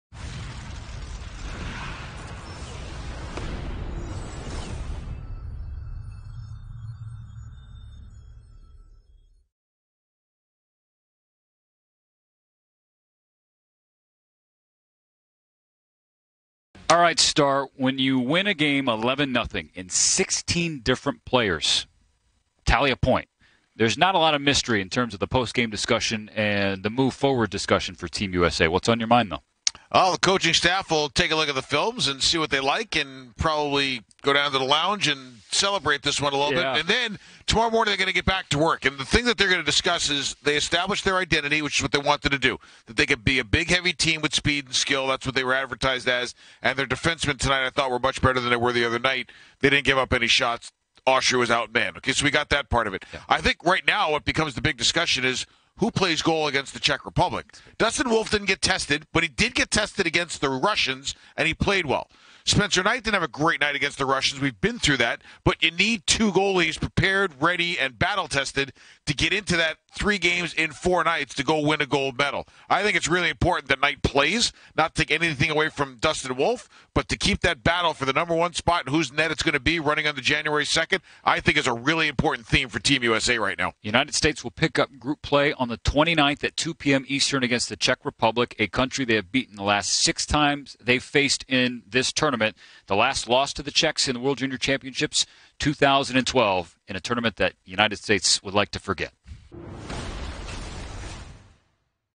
Team USA's Win Over Austria and Moving Forward | 2021 World Junior Championship
Stephen Nelson and Dave Starman discuss Team USA's victory over Austria and what's coming up next for them in the tournament.
2021 IIHF World Junior Championship WJC